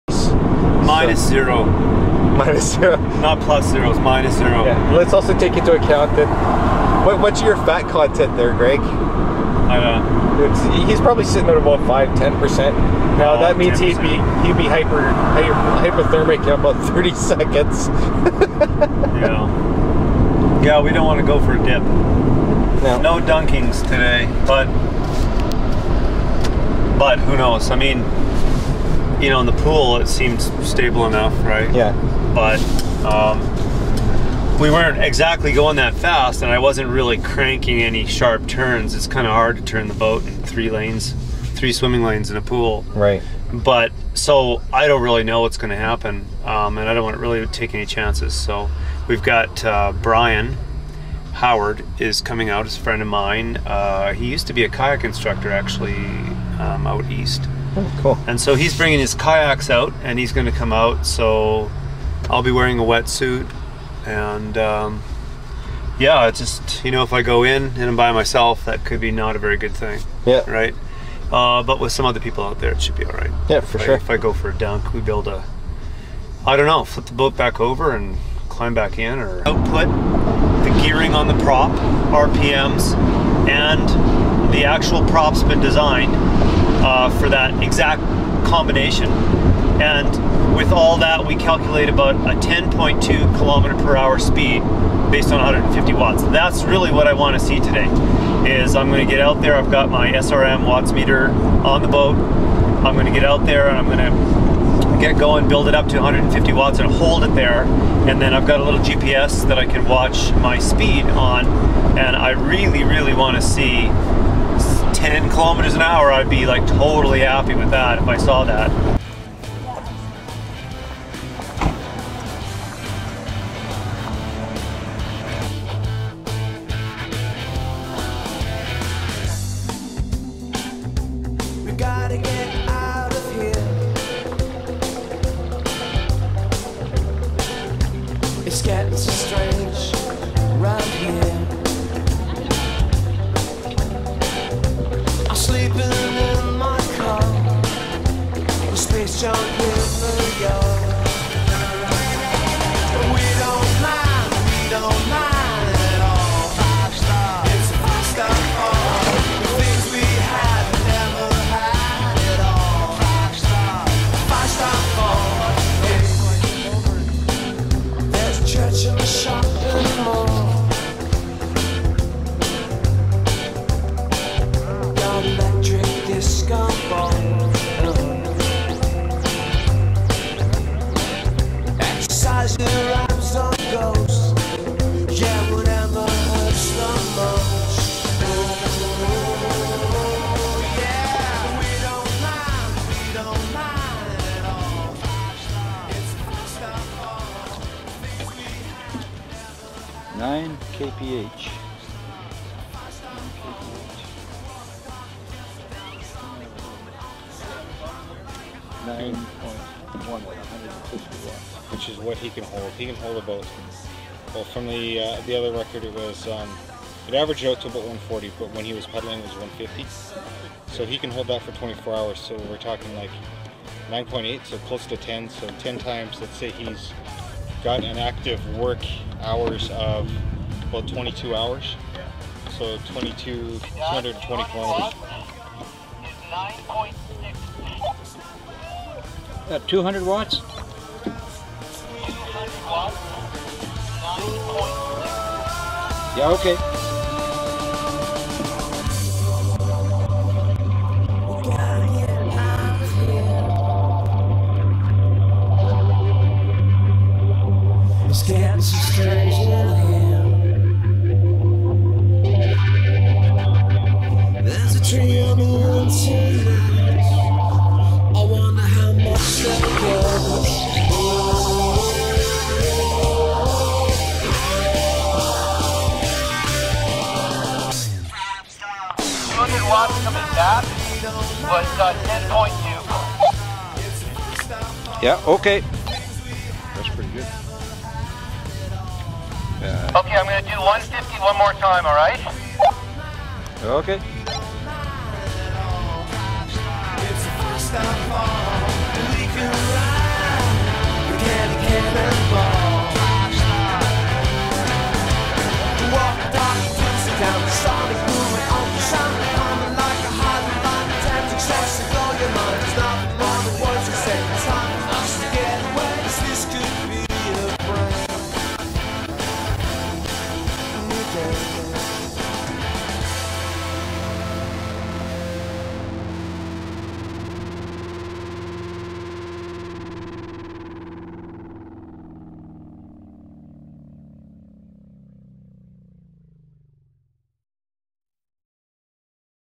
WiTHiN Human Powered Boat Lake Test
Greg Kolodziejzyk tests his human powered boat WiTHiN-24 at Glenmore Lake in Calgary